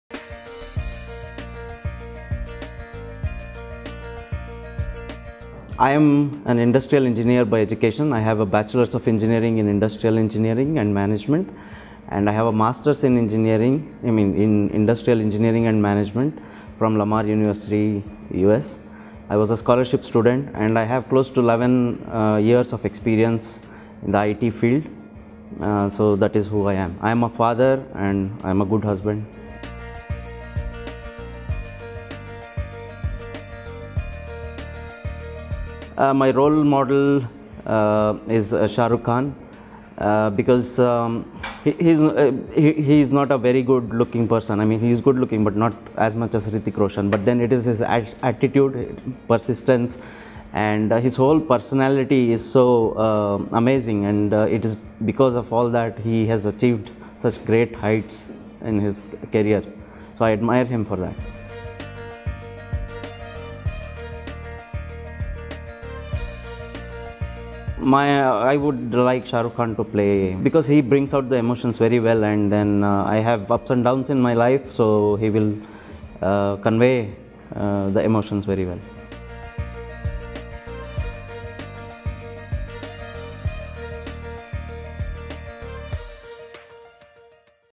Venkatesh Ranganath - EMBA Class of 2017-B
Venkatesh Ranganath, a student of the EMBA program at the Institute of Product Leadership introduces himself as a person, his role model and top reasons for joining the program.